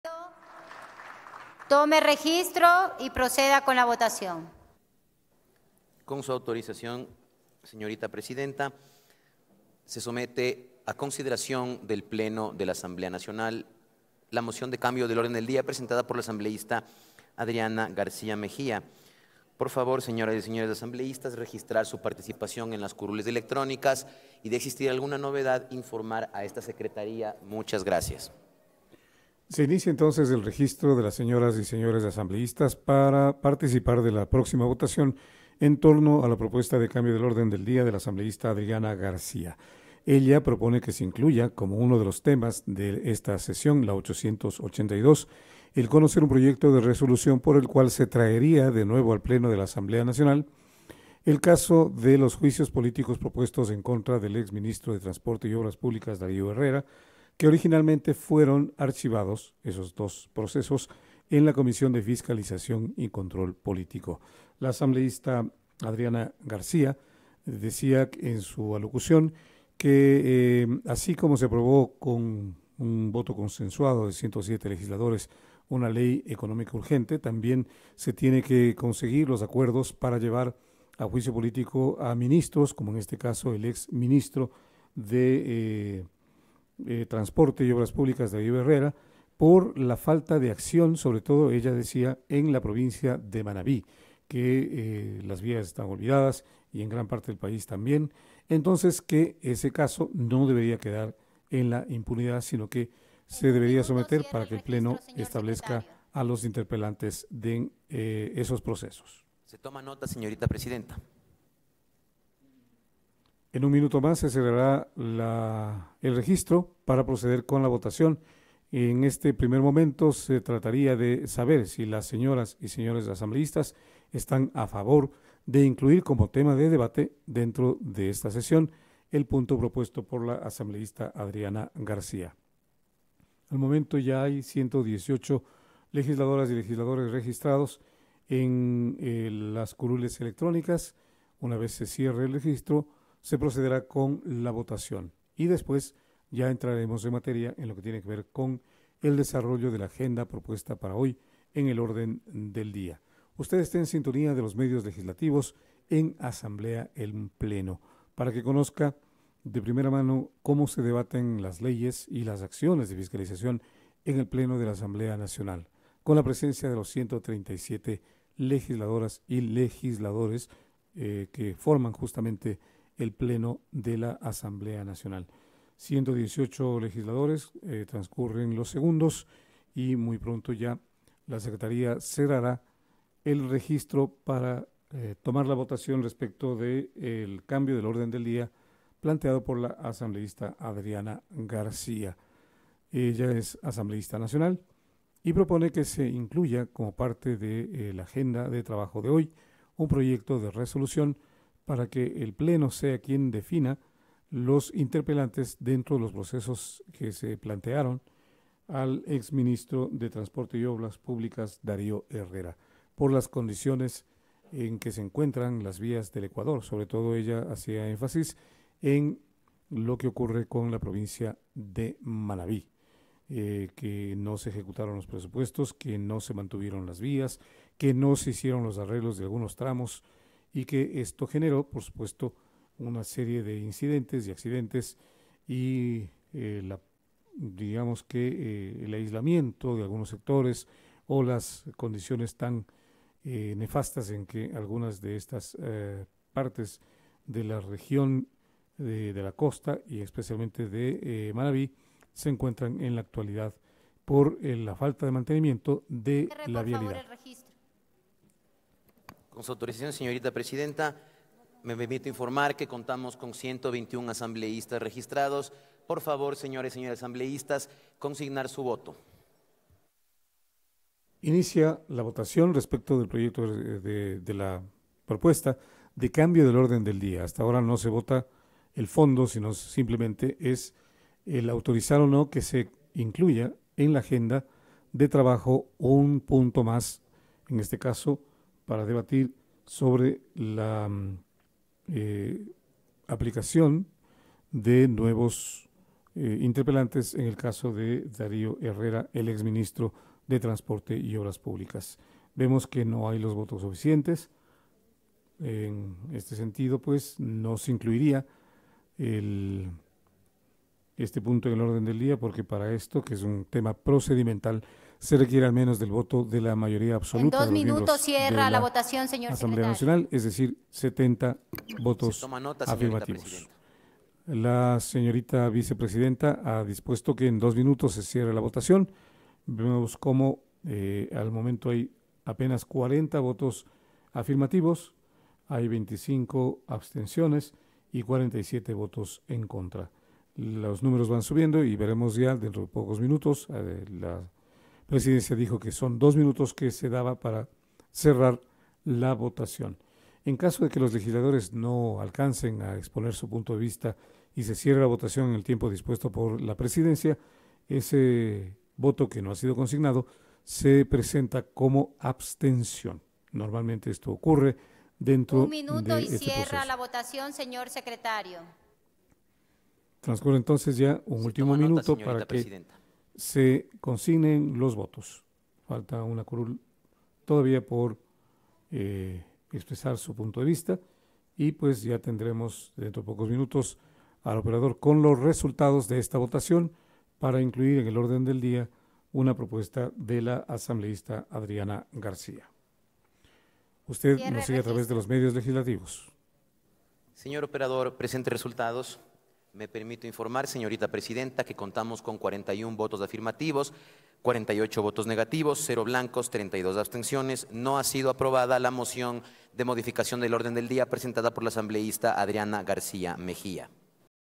Votación de la moción de la asambleísta Adriana García - Sesión 882 - Cambio del orden del día
21 de diciembre de 2023
Mocionó traer al Pleno al exministro de Transporte Darío Herrera para su fiscalización.